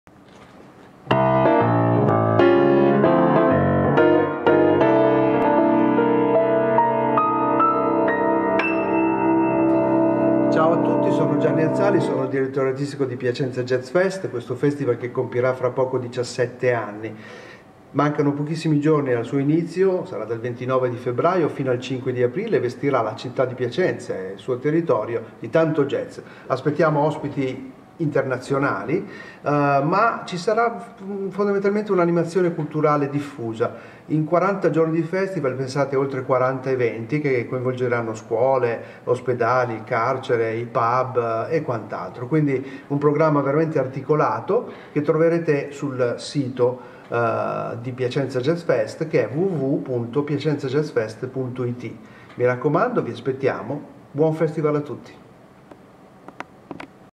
PcJazzFest: l'invito di Gianni Azzali
Il direttore artistico del Piacenza Jazz Festival di Piacenza presenta l'edizione 2020